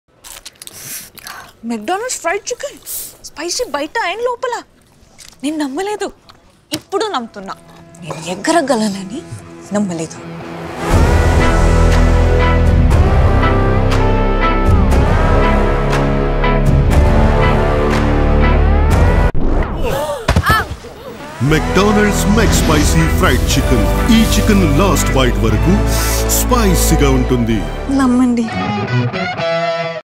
McSpicy Fried Chicken - McDonald's India | Rashmika Can Fly...Or Can She? (Telugu)
The spiciness of McDonald’s McSpicy Fried Chicken is making Rashmika do unbelievable things.

*Available across select stores in South India